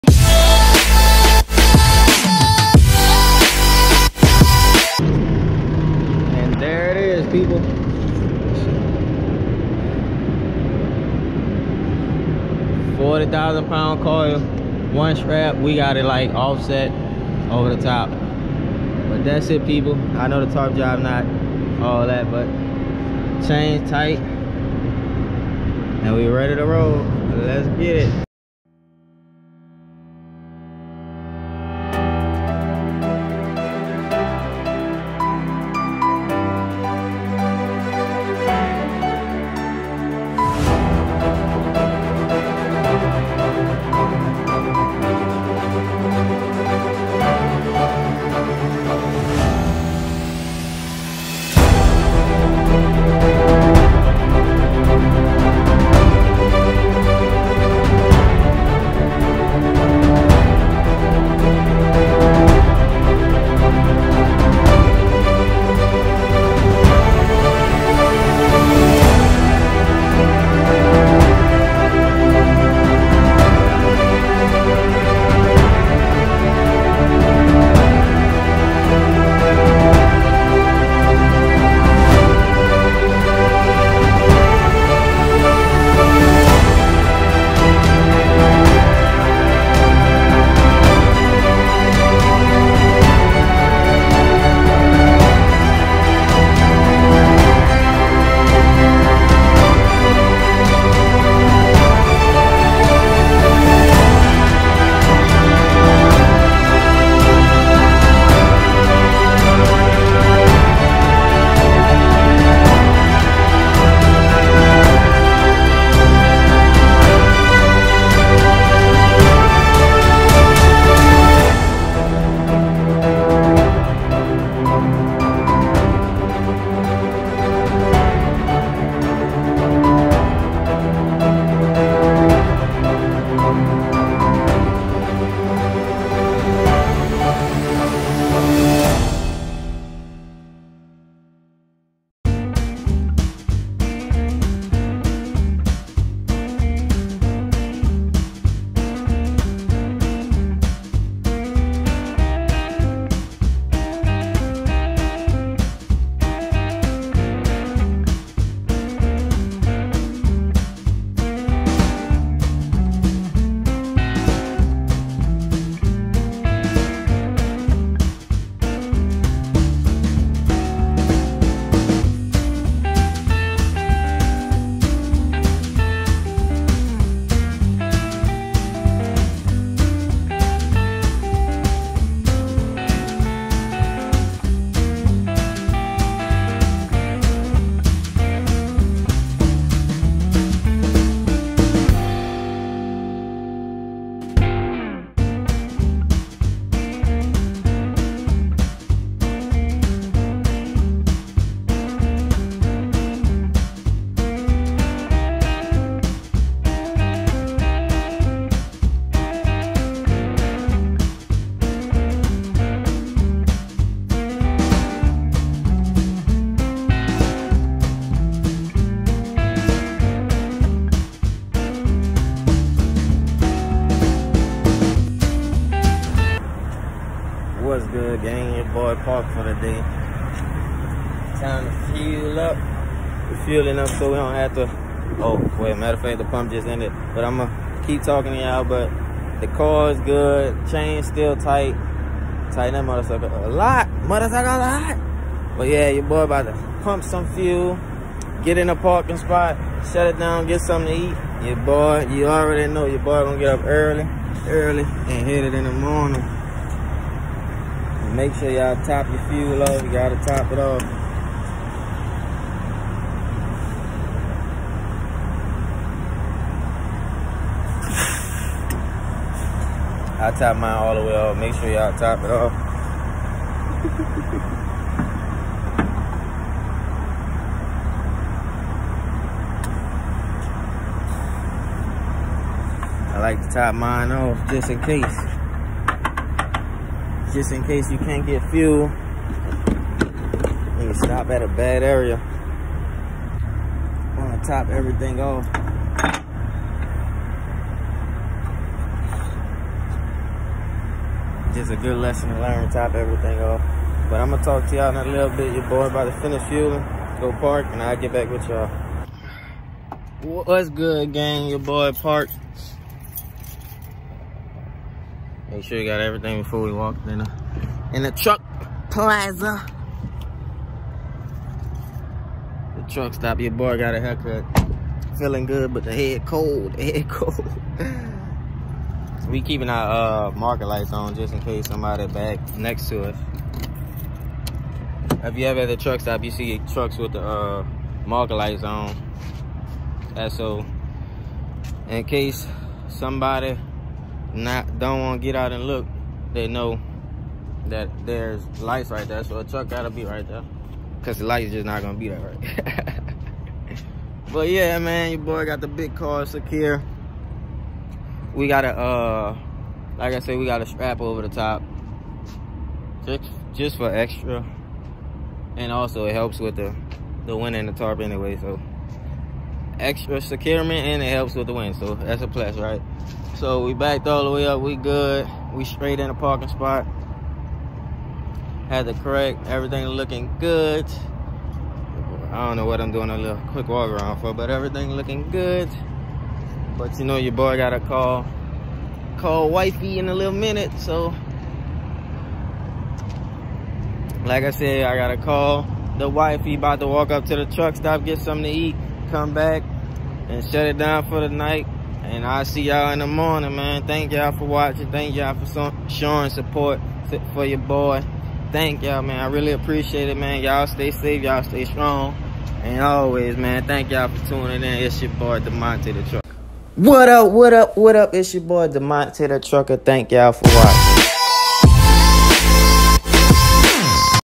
Heavy Coil Montgomery Transport The Life of a Flatbed Truck Driver Vlog
Welcome back ppl, were in mt Vernon Alabama picking up a heavy coil